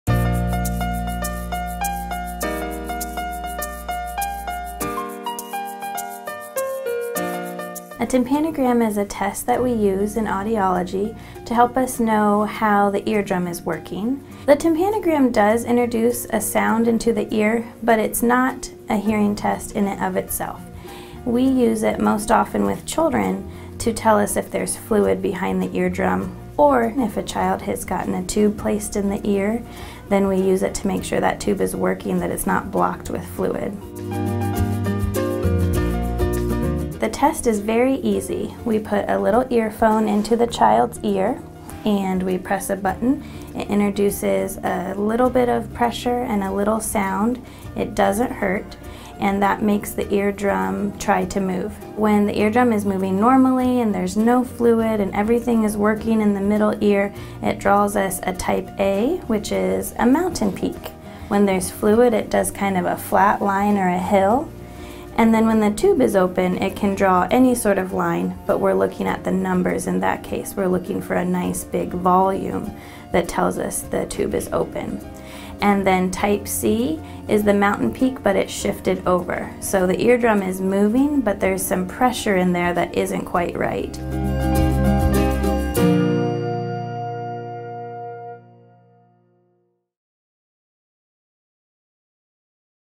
Tympanograms - Boys Town National Research Hospital
Tympanograms are commonly used to test the condition of the middle ear and ear drum. Kendell Simms, Au.D., CCC-A, audiologist at Boys Town National Research Hospital, explains how a tympanogram works and how to understand the results. For more information on our Hearing & Balance services,